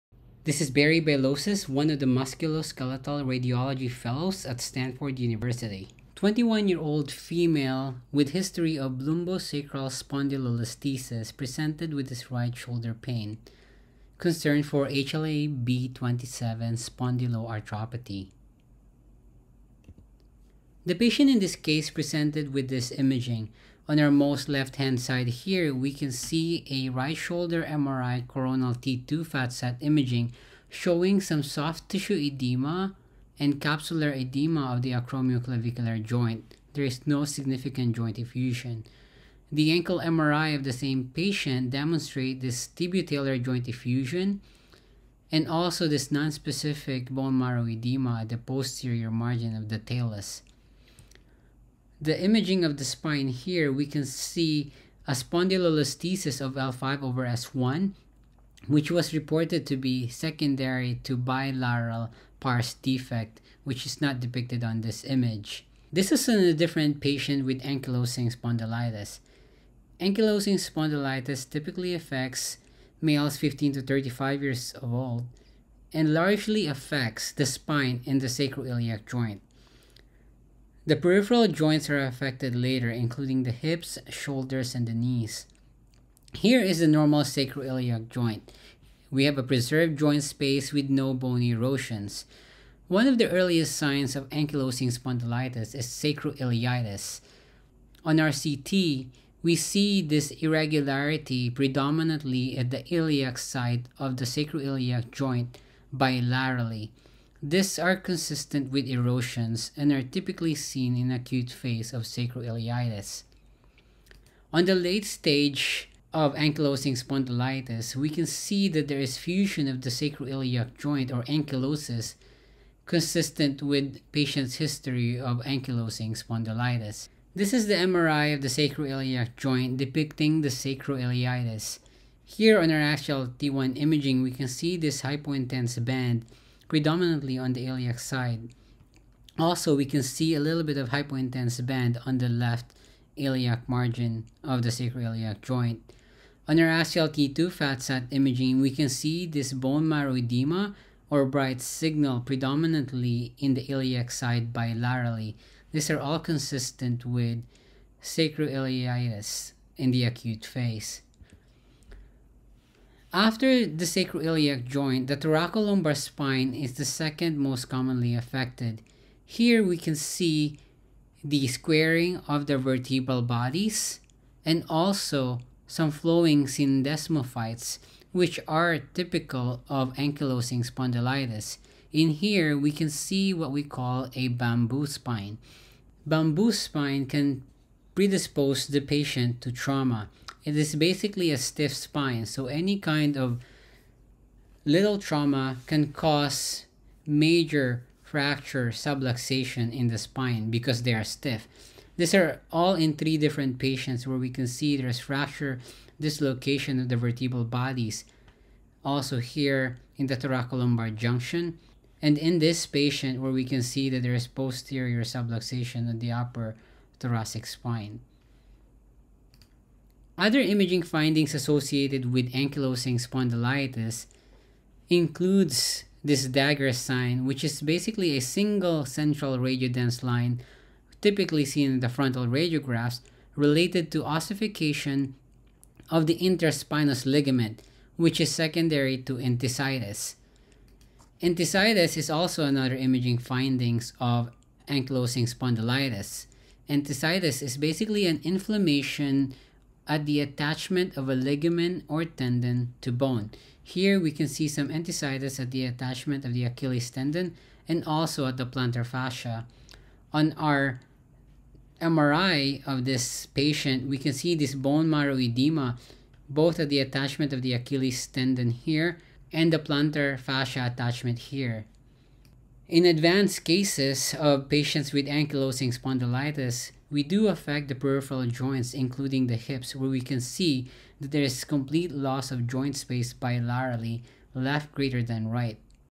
Sports Medicine Case 46: 21-year-old: Tennis-Synovitis: Shoulder Pain and Finger Swelling
Seropositive spondyloarthropathy